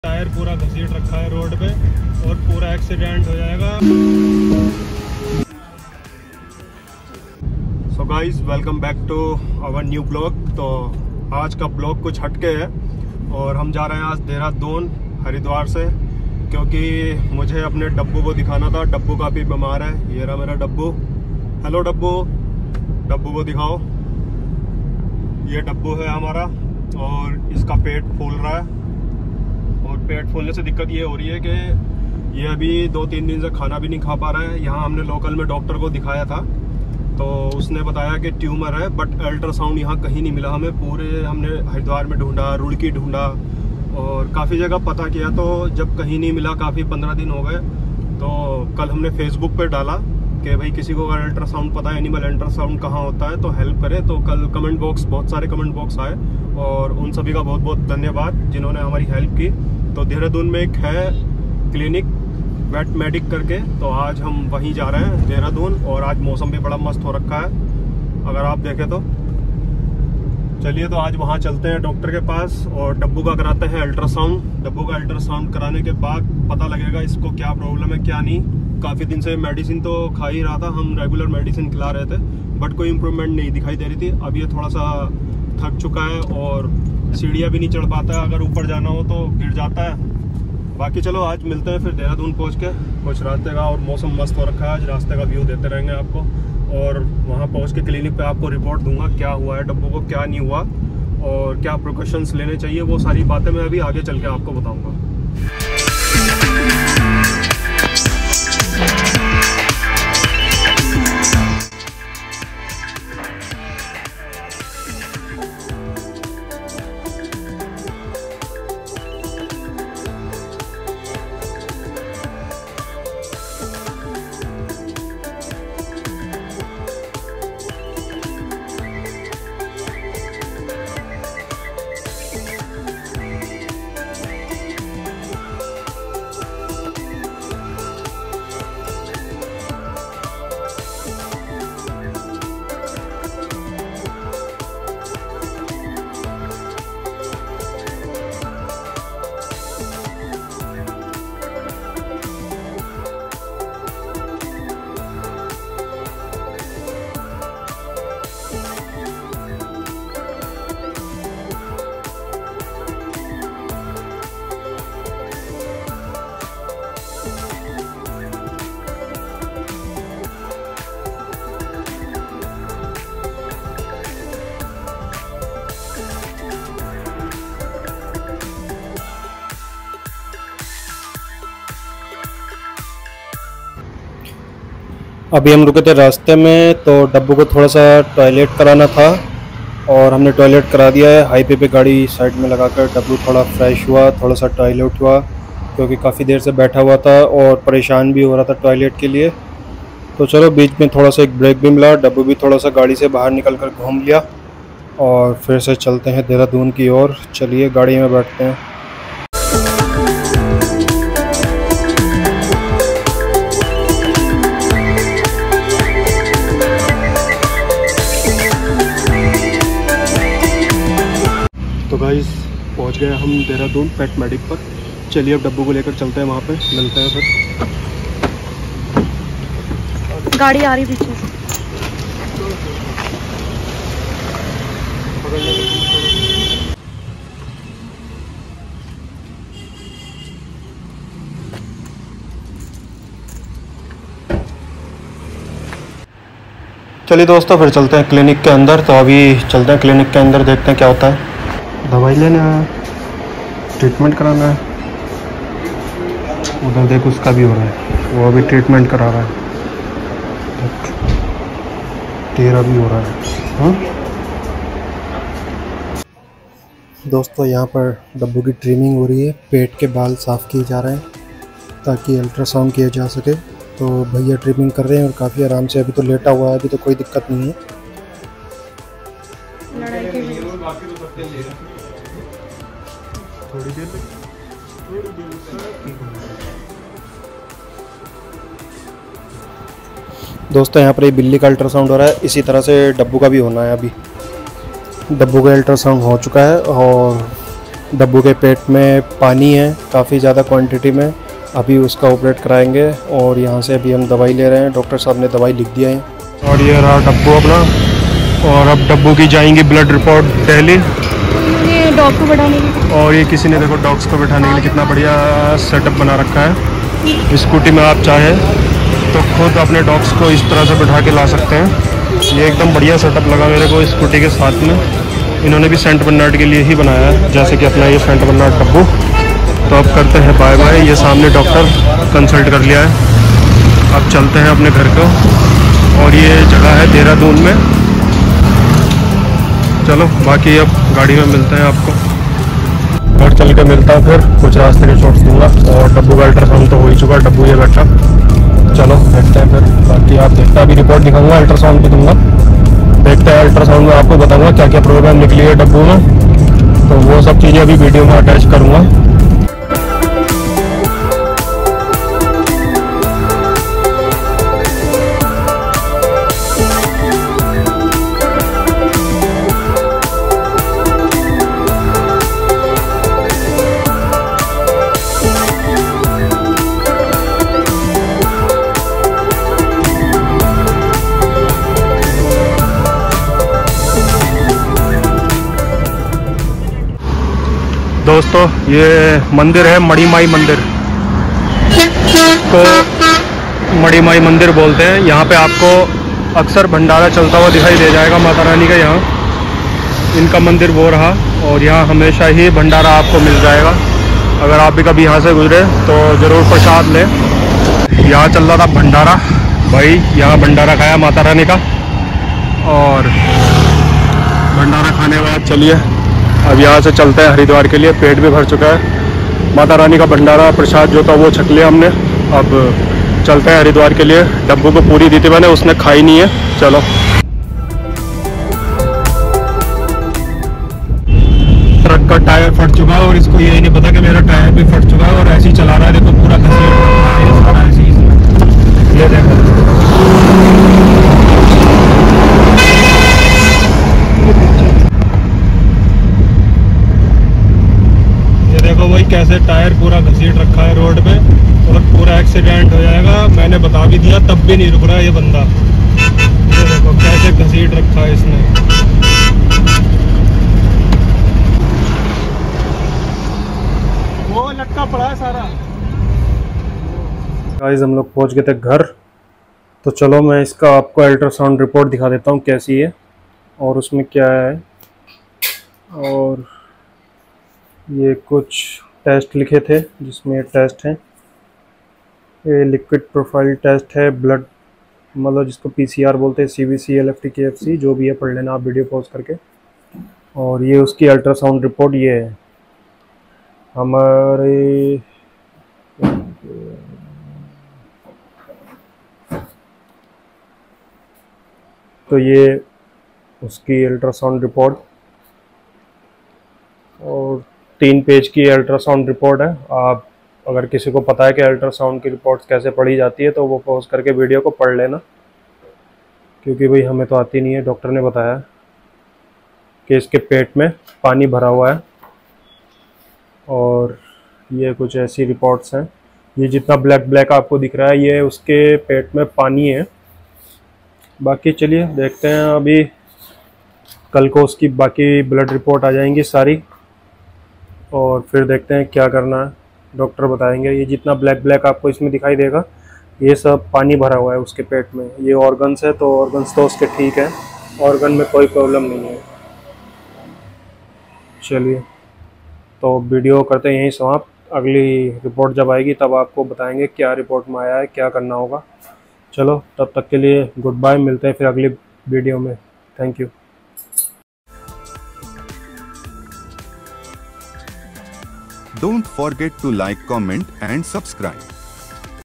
Dog ke pet me paani bharna|Dabbu ko Ascites ho gya pet me|ultrasound,treatment
Hamare dabbu ke pet me paani bhar gya or pet fulta ra rha tha tabhi hamne iska ultrasound kraya usme pta lga ke paani bhar chuka hai pet me. Is bimari ko ascites bolte h. Ab iska treatmenton medicine se hoga.